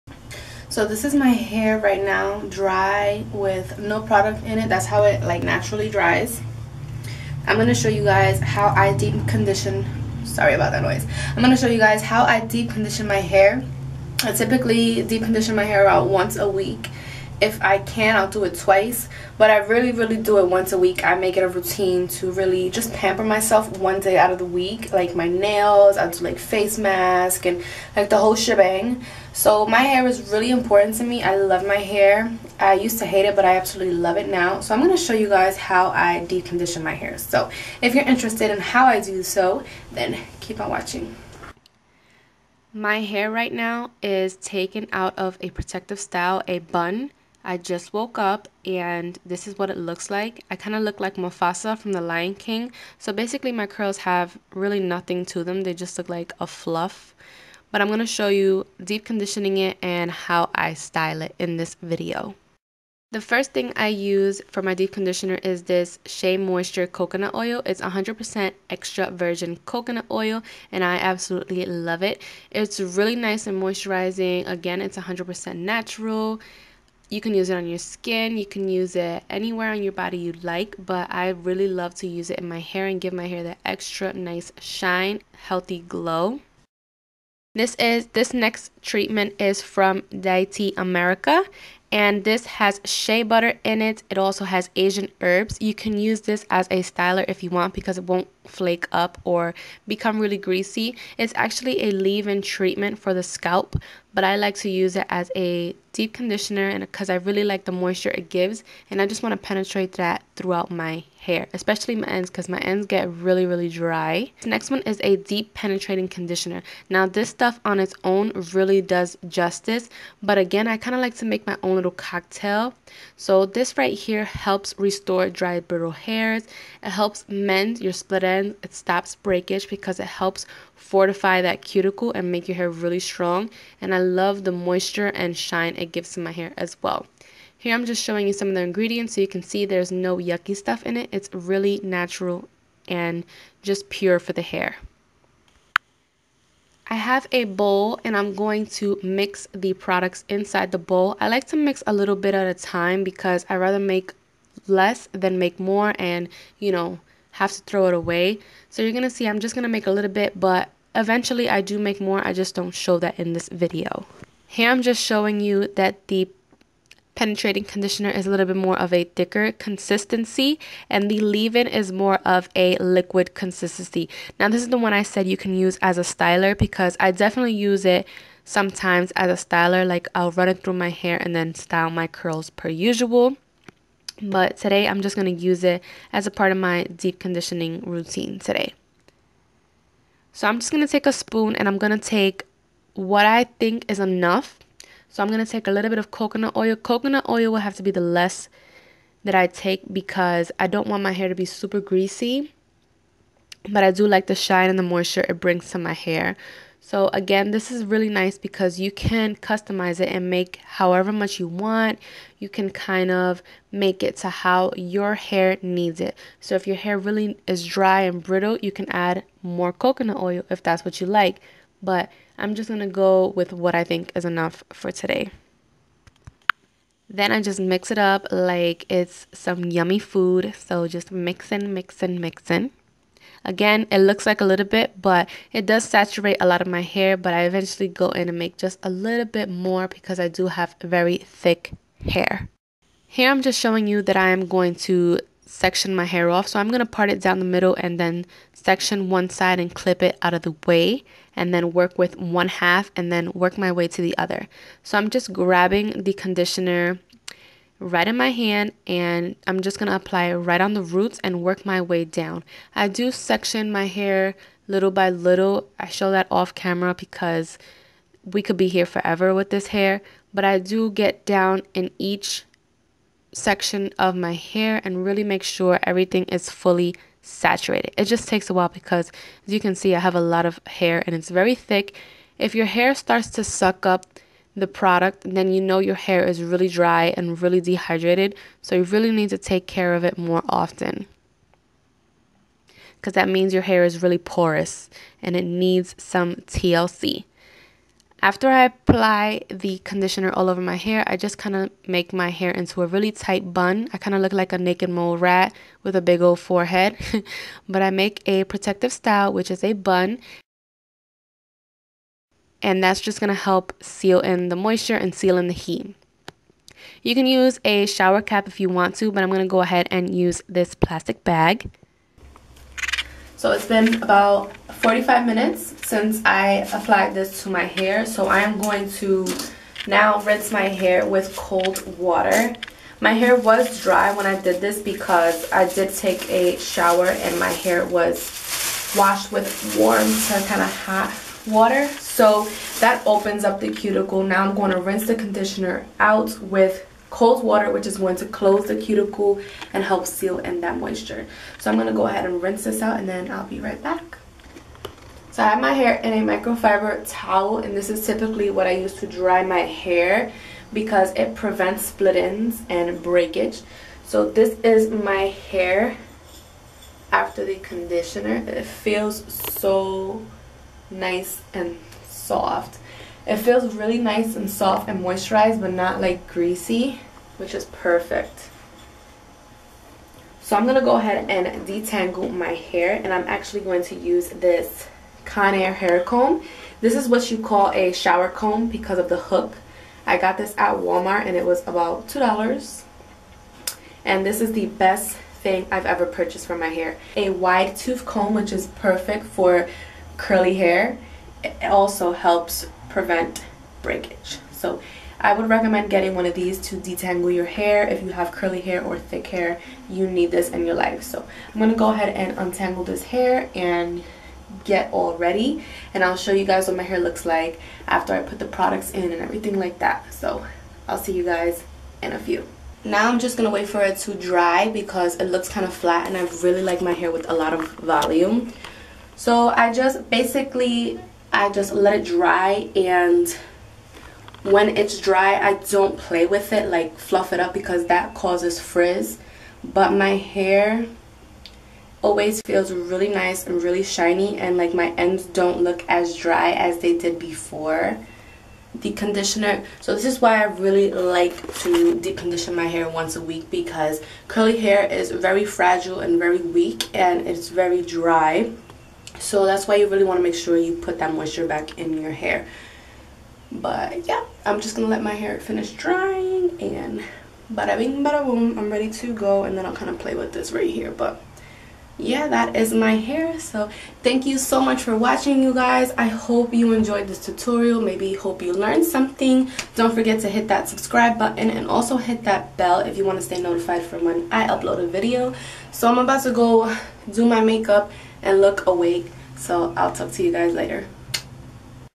How I DEEP CONDITION My Curly Hair| Vlog style
Hey beauties ( Do not forget to watch in HD)

   Here is how I have been deep conditioning my hair and I love it so so much. You really need to try these products from Deity America they are amazing. I hope you all enjoy this quick video please let me know how you manage your curly hair.

TO PURCHASE A SHIRT CLICK THE LINK IM BIO- (Use code ASHLEYMUA)

Coconut oil-
Walmart Comb-

Ashley